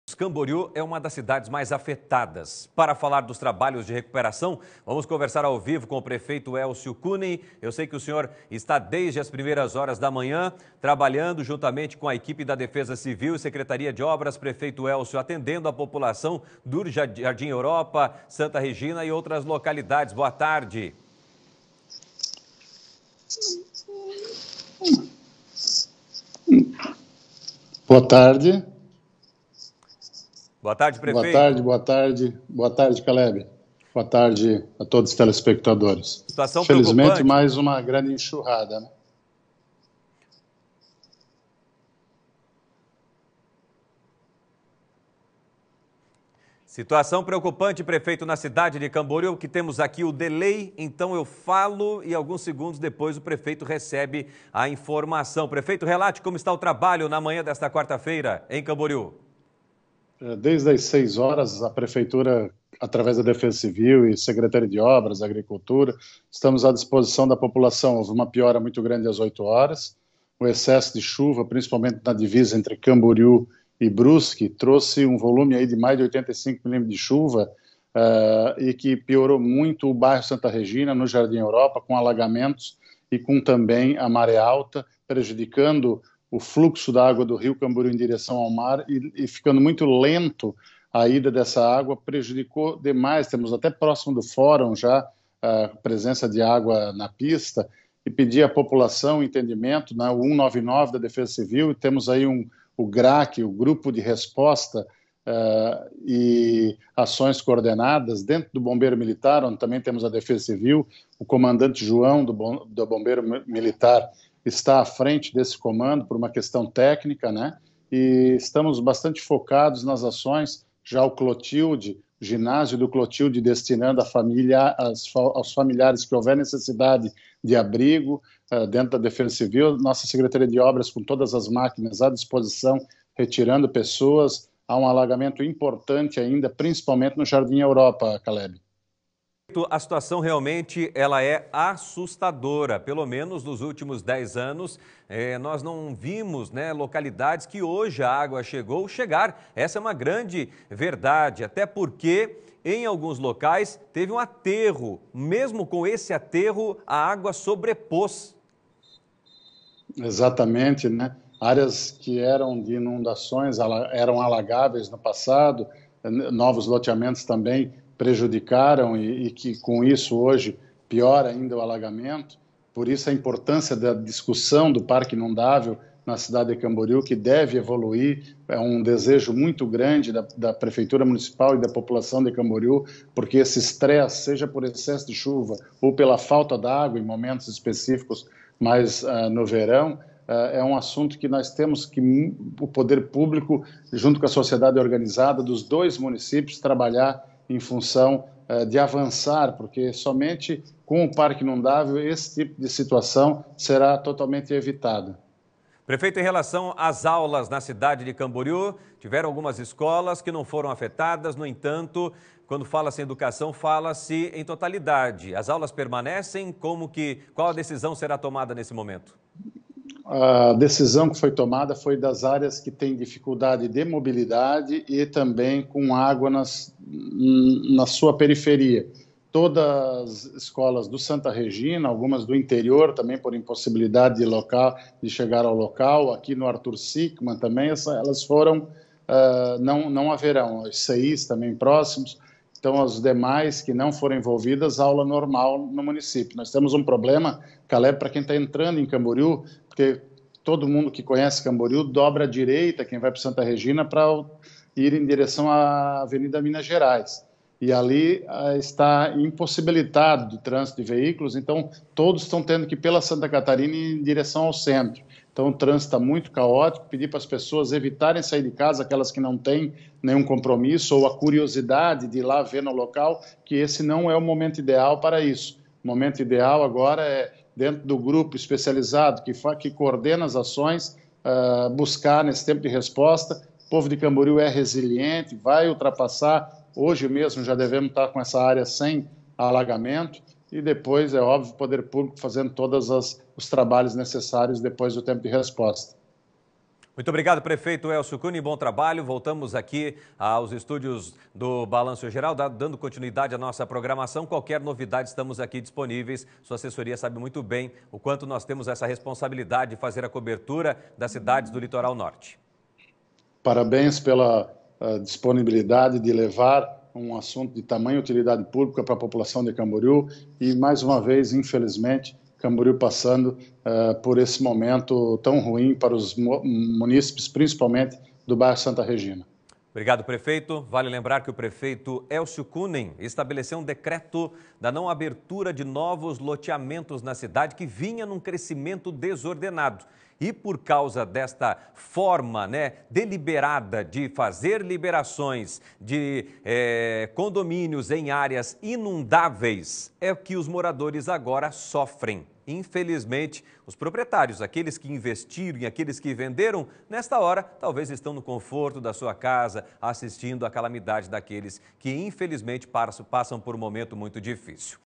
Chuvas causa estragos em Camboriú
Camboriú foi umas das cidades mais afetadas pelas fortes chuvas